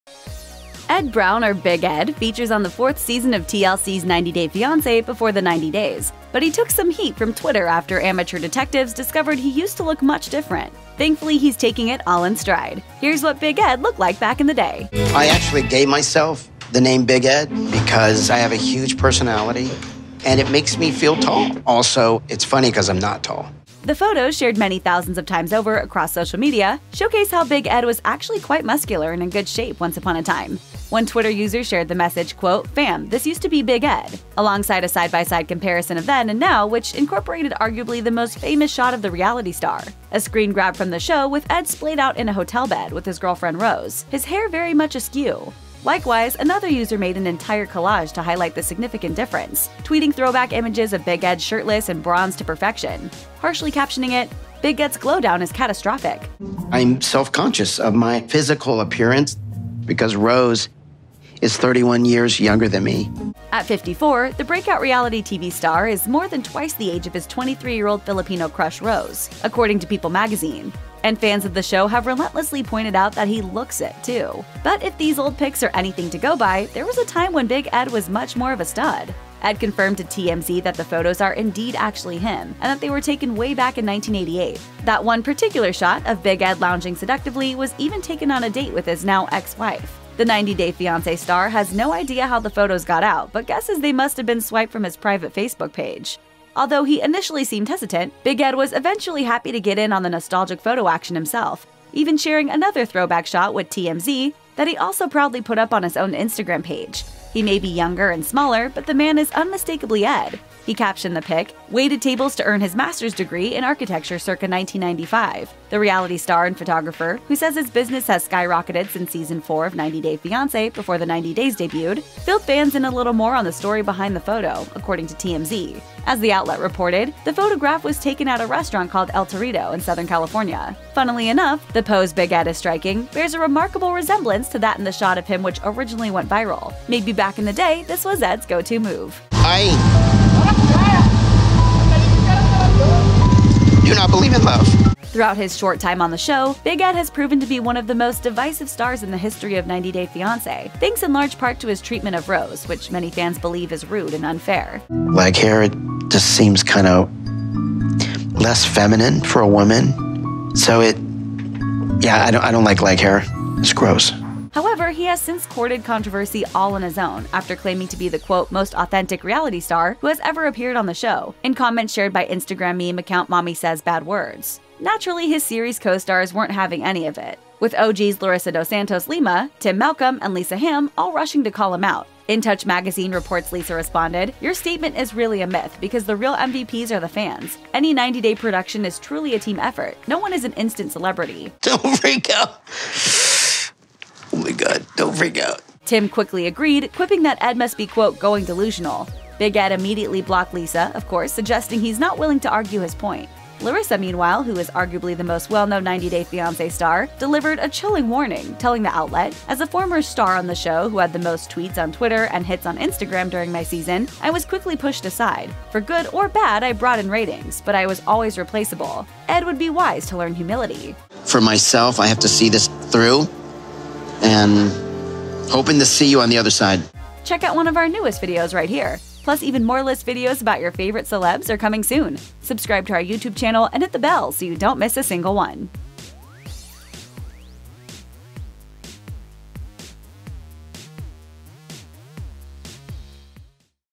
90 Day Fiance's Big Ed Is Unrecognizable In Throwback Photos
“Big Ed” Brown is one of the most infamous people to ever appear on the TLC reality show 90 Day Fiancé, as his treatment of his much younger fiancée Rose earned him much ire from fans. However, Big Ed has now gone viral for a less-awful reason, as people have discovered some throwback pictures of the man, and they are pointing out how good he used to look. The reality star has confirmed that the photos are of him, even though he doesn’t know how someone got a hold of them. Let’s take a look at the unrecognizable throwback photos of 90 Days Fiancé’s Big Ed.